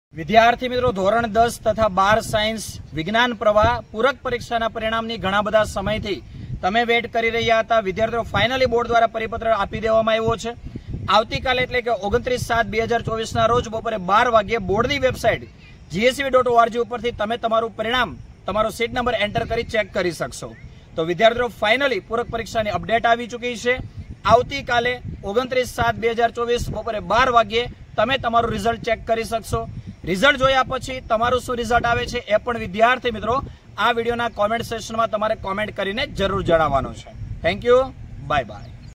ધોરણ 10 તથા 12 સાયન્સ પૂરક પરીક્ષા નું પરિણામ જાહેર ચેક કરો આવી રીતે | Improvement Exam 2024 Result
To Join NEET 2025 Dropper Batch Online & Offline all Subjects